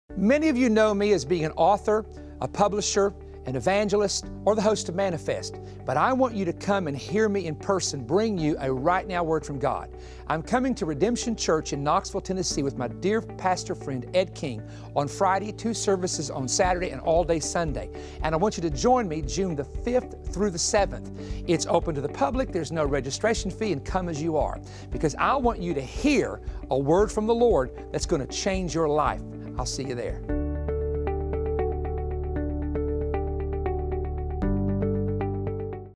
Perry Stone Regional Conference at Redemption Church
Join us for a 3 Day Prophetic Event!

Friday - June 5 / 7PM
Saturday - June 6 / 10AM & 6PM
Sunday - June 7 / 10:30 AM & 5PM